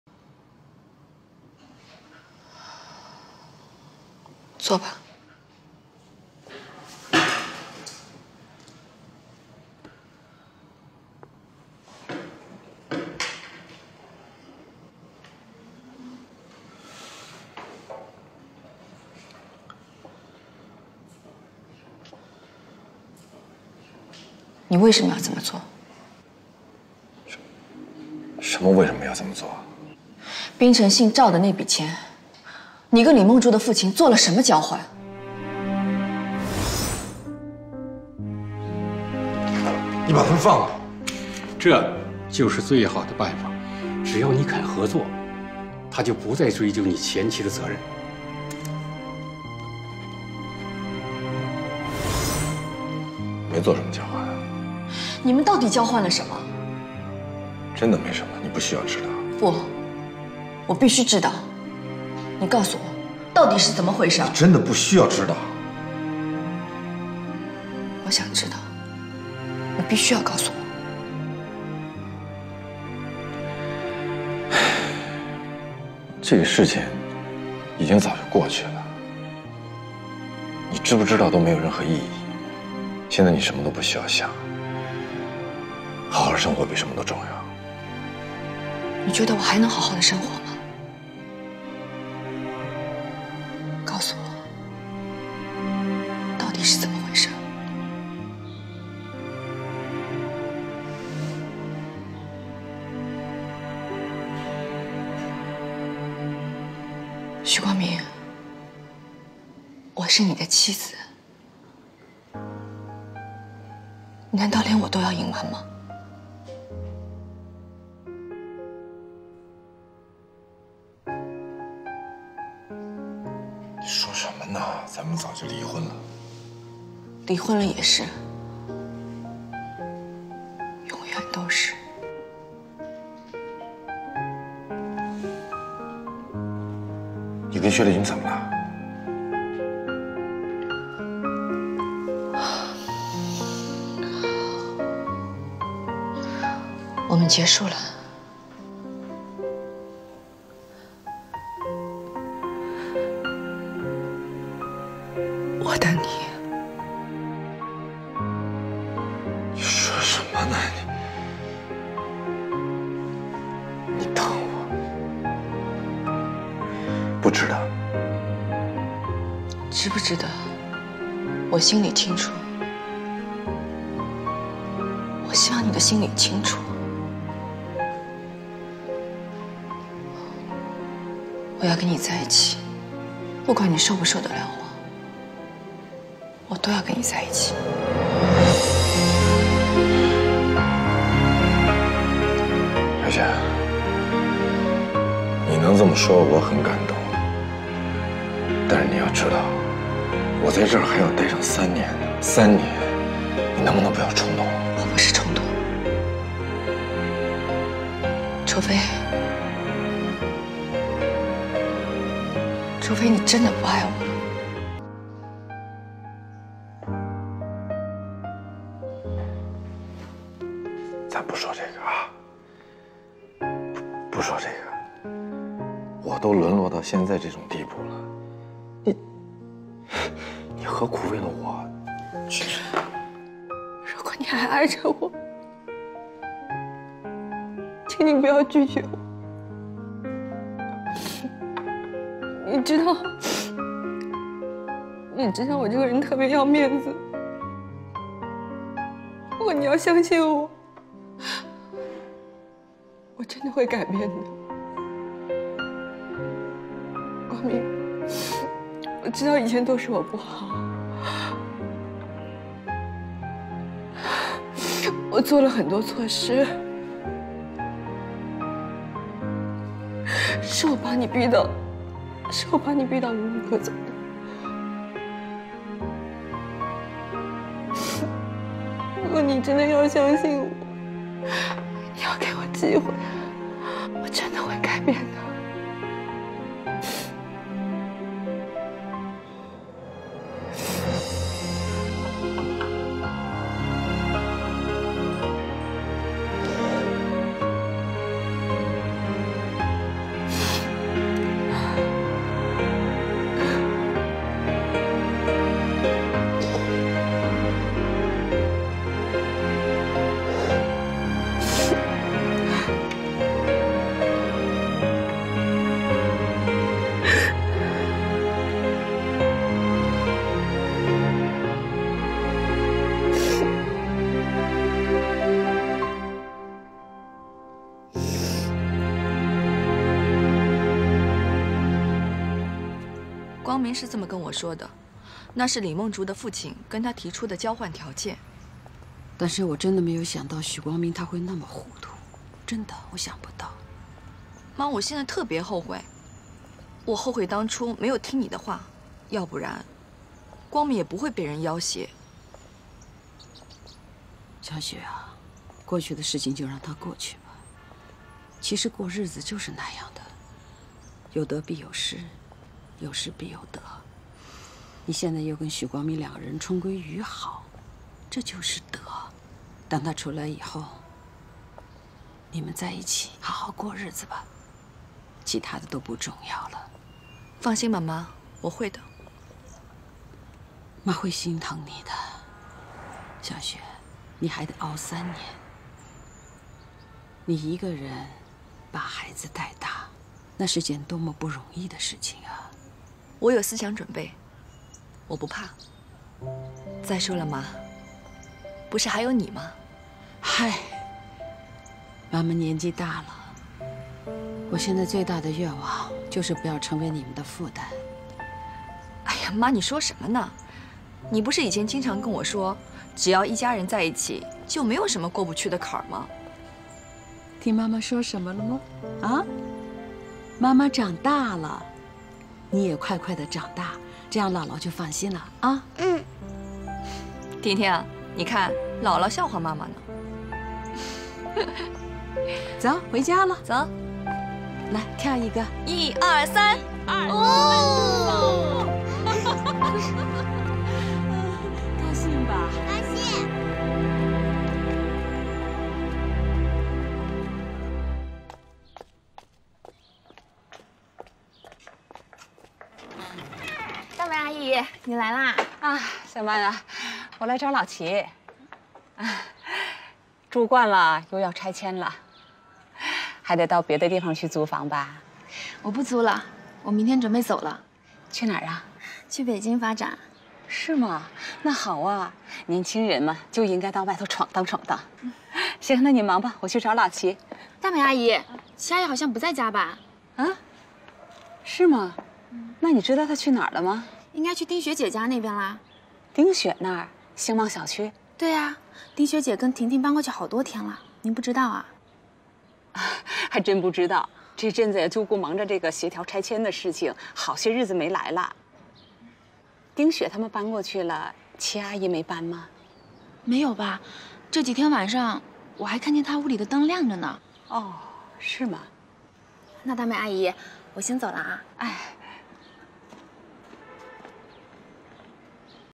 得知前夫為自己所做的一切，前妻感動痛哭，發誓不再嫁人等他出獄！
"37-wmda
💘【劇情簡介】該劇以都市生活為背景，講述了一個家庭三代人（靳東 童蕾 潘虹 領銜主演）之間關於親情和責任的故事